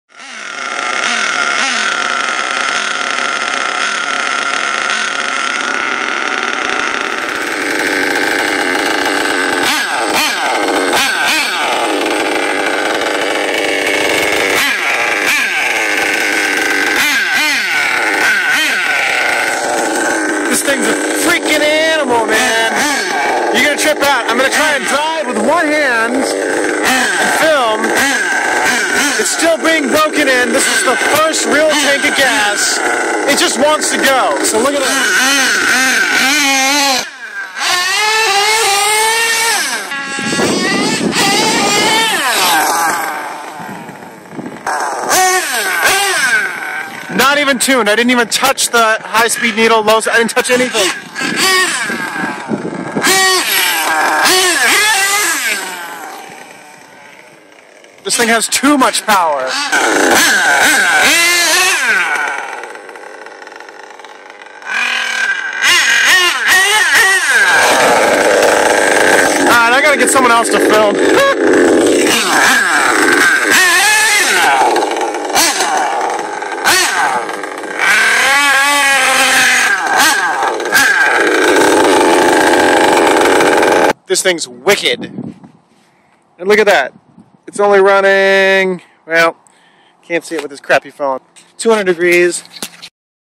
$4,000+ HPI Savage w/ Novarossi, popping wheelies and shifting through the gears
FIRST TANK OF FUEL, NOT TUNED! See the full build up thread here!